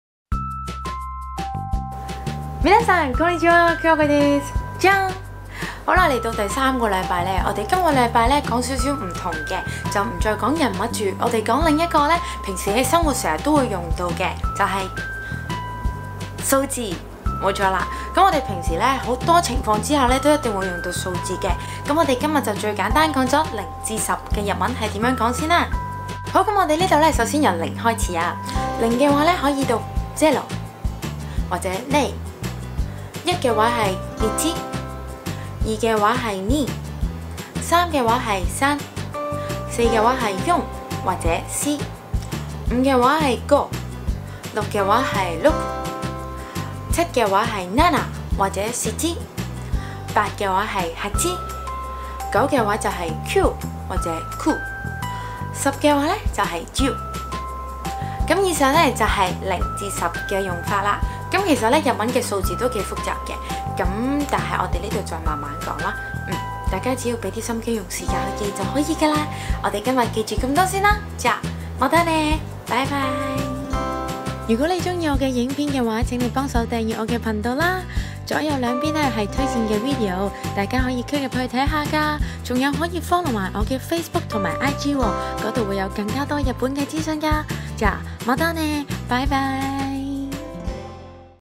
日文 香港 日常單字篇: 學識0-10 可以去日本購物啦~ #5 [黑貓響子]
日文的數字都幾多變化同複雜
當初響子都花了好多時間~~
但初學的朋友可以先記下0-10的說法~~
超好用唷~~~^-^